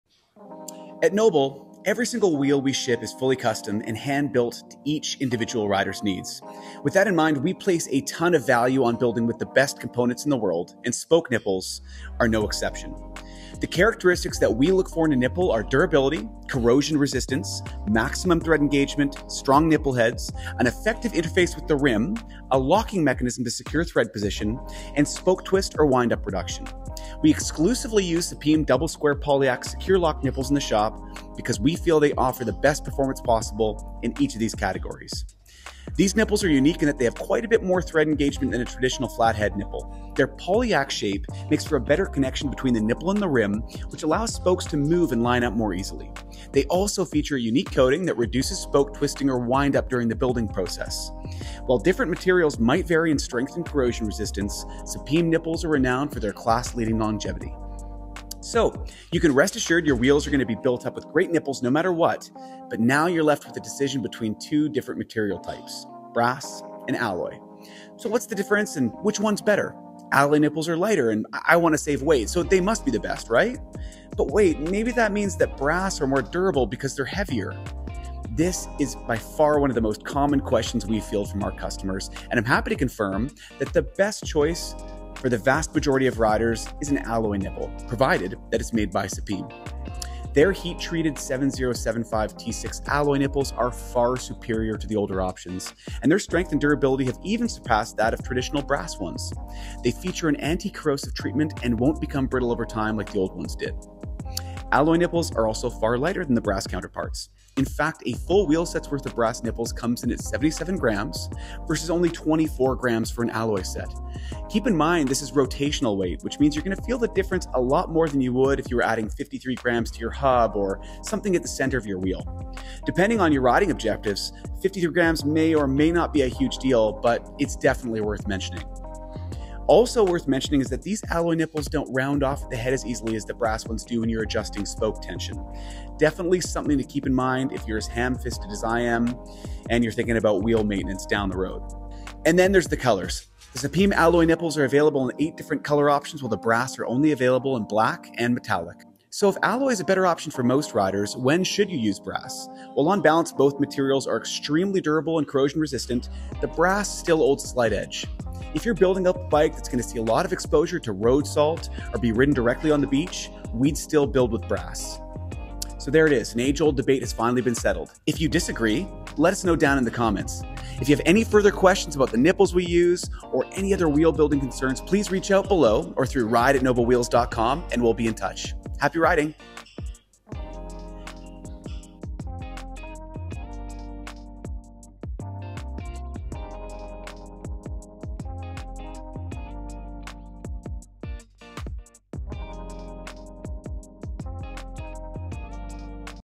Brass VS Alloy Nipples
We look at the difference between brass and alloy nipples? Why are there two material types, and which one is better?! It’s a question as old as time, or at least as old as the bike wheel.

More From NOBL!